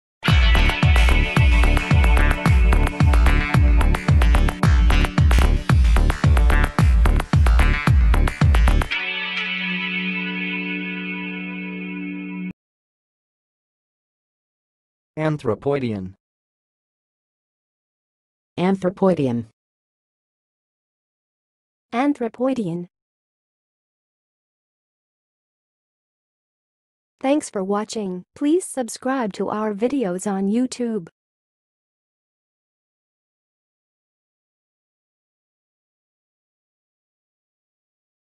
How to say "anthropoidean"! (High Quality Voices)
Watch in this video how to say and pronounce "anthropoidean"!